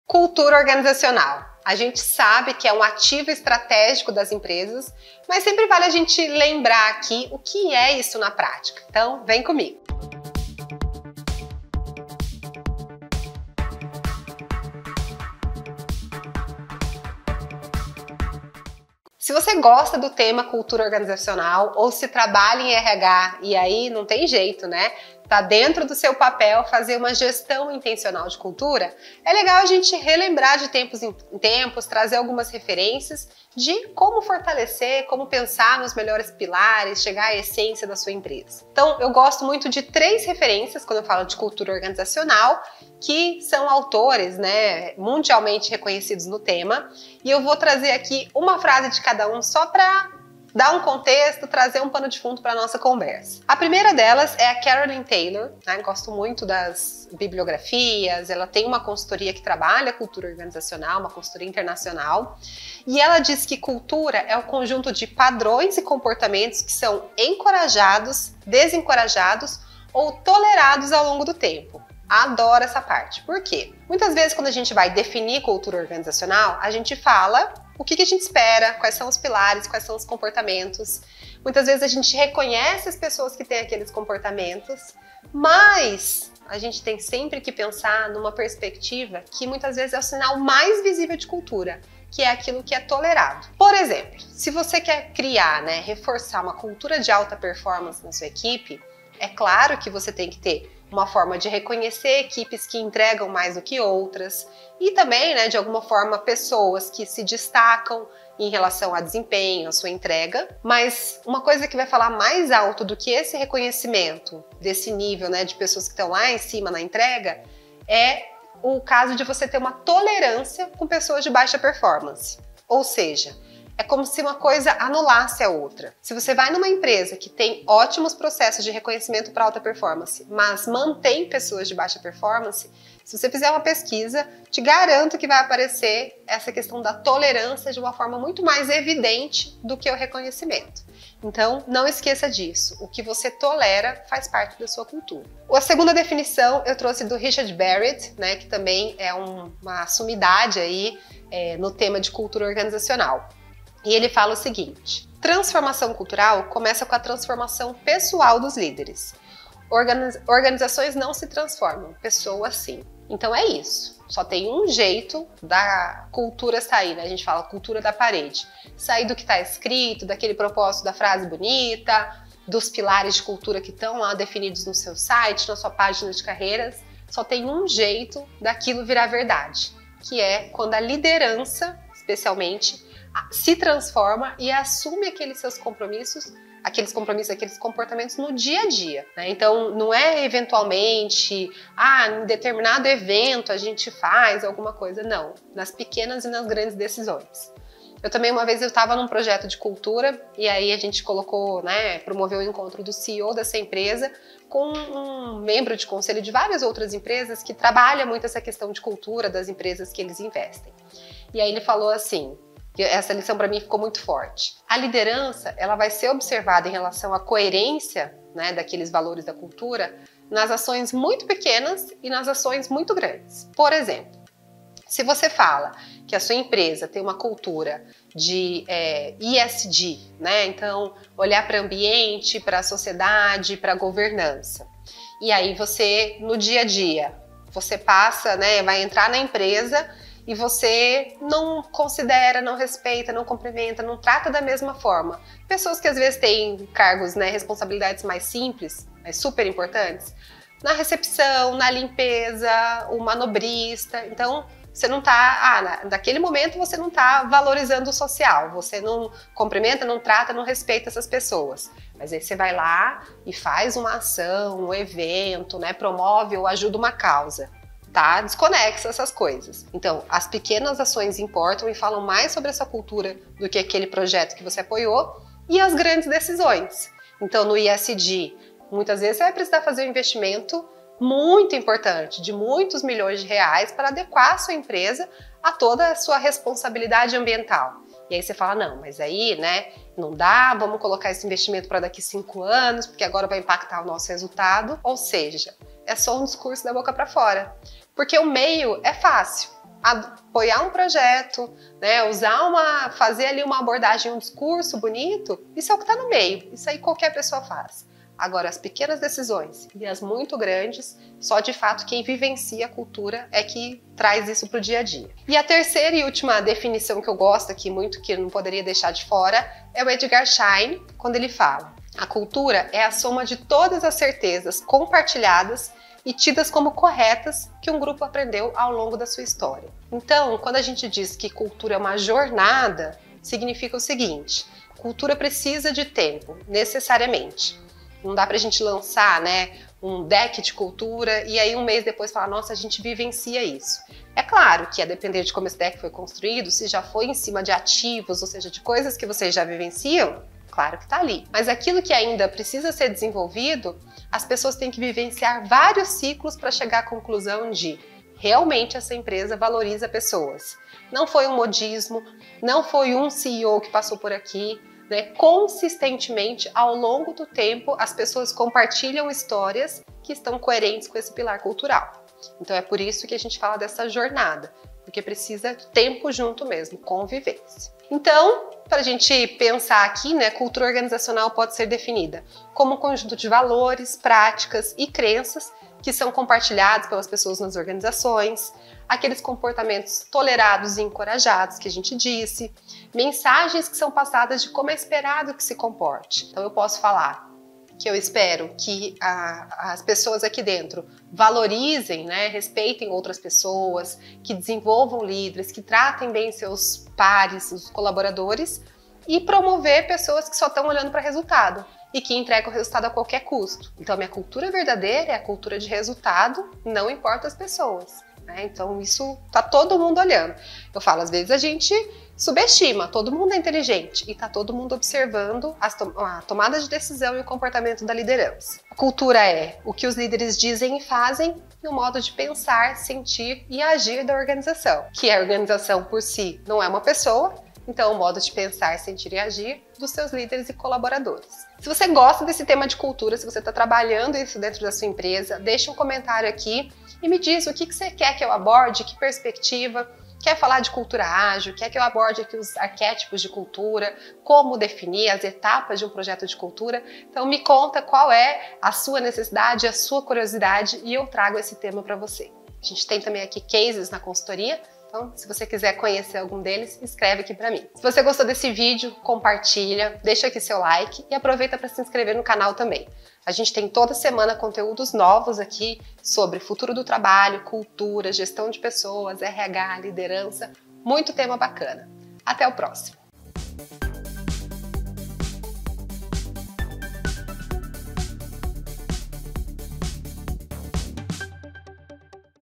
Cultura Organizacional: Você Sabe o Que É (Mesmo)? - Laura Widal
Cultura Organizacional: Você Sabe o Que É (Mesmo)?

O tema cultura dentro das organizações tem se tornado cada vez mais relevante, os profissionais de RH enfrentam desafios para conseguir implantar o conceito e que todas abracem essa causa. Mas afinal, o que é cultura organizacional?

Neste vídeo vou compartilhar com você trechos de autores para exemplificar e explicar o que é cultura organizacional de forma descomplicada.

Se você ainda tem dúvida sobre o que é cultura organizacional e como fortalecer o conceito dentro da sua empresa, vem comigo!